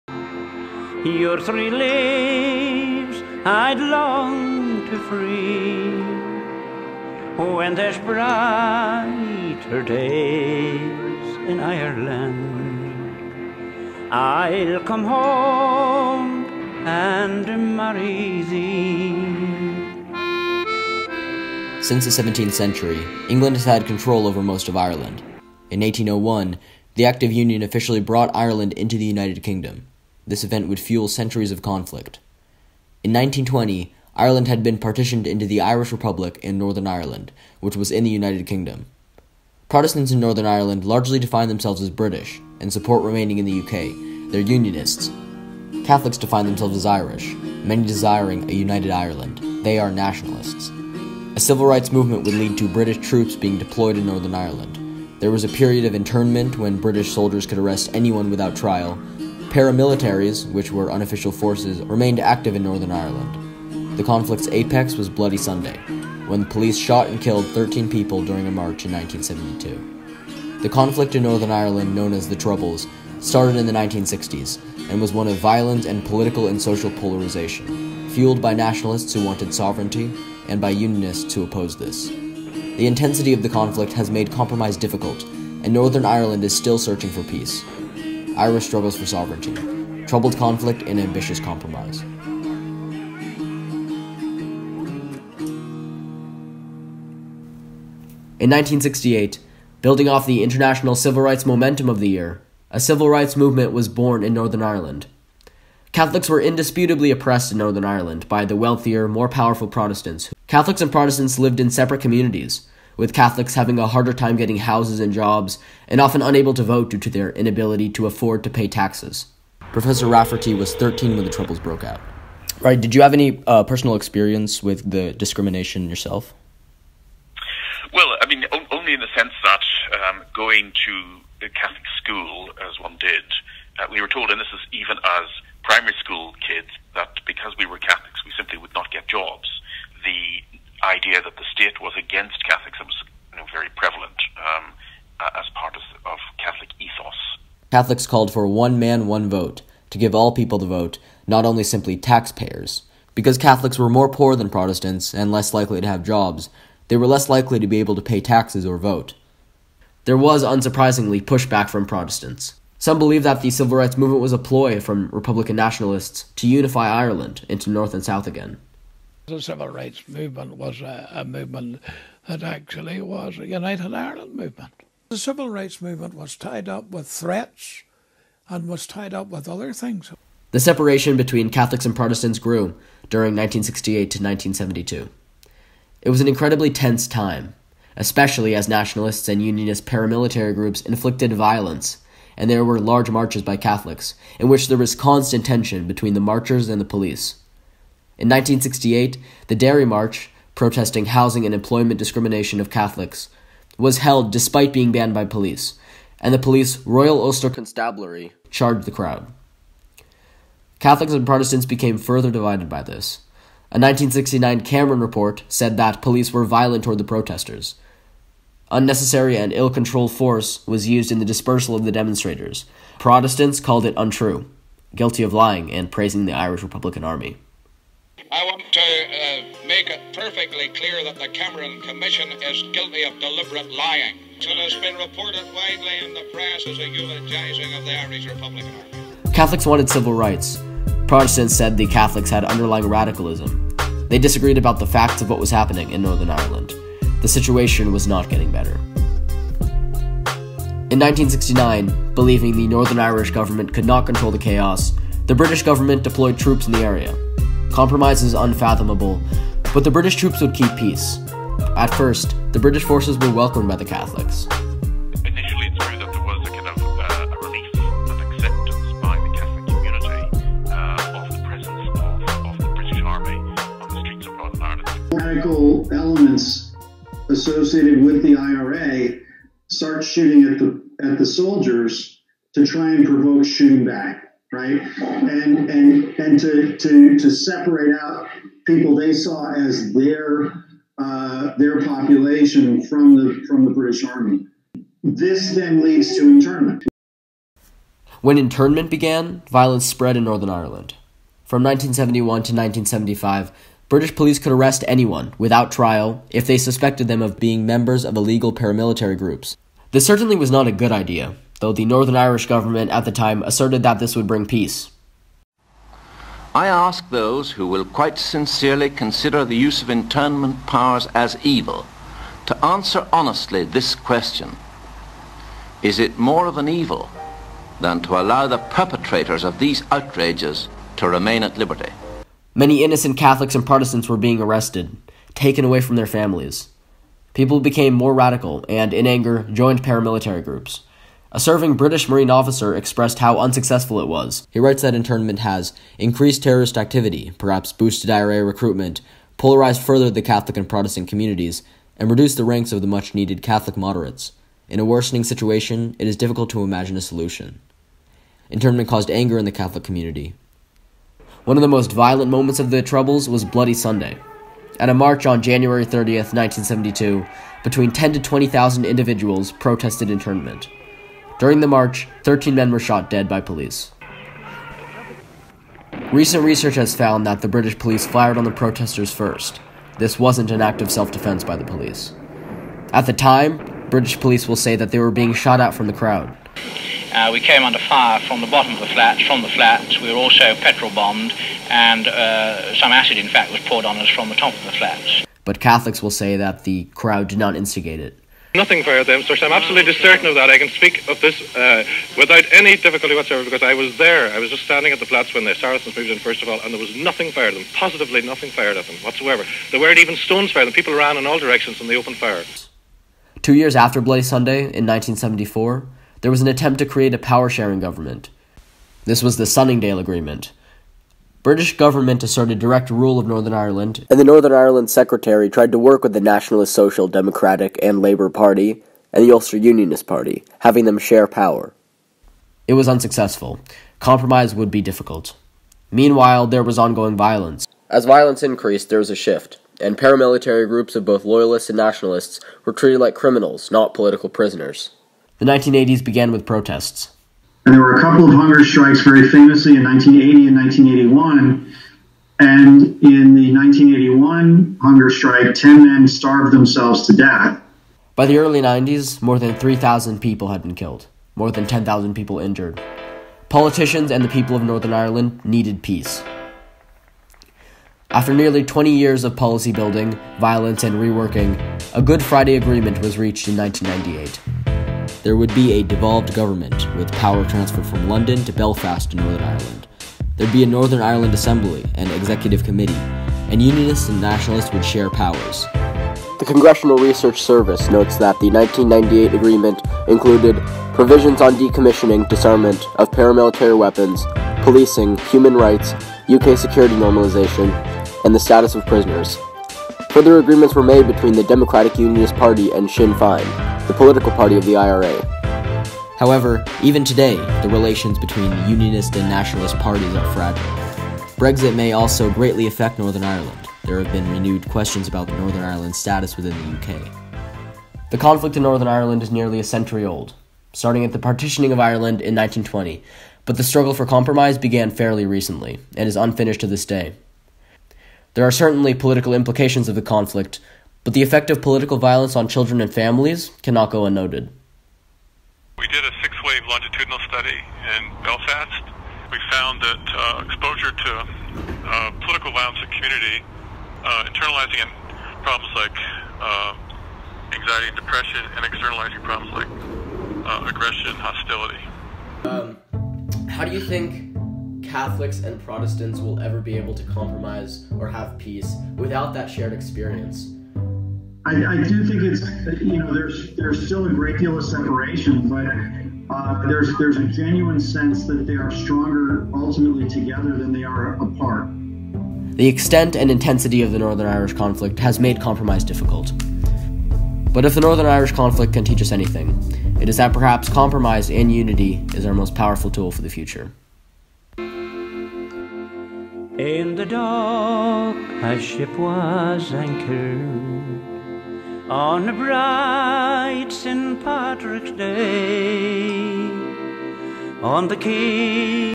The Troubles of Northern Ireland in 10 Minutes (2018)
Short documentary on the "Troubles" of Northern Ireland, competed in the Los Angeles National History Day Competition under the theme "Conflict and Compromise."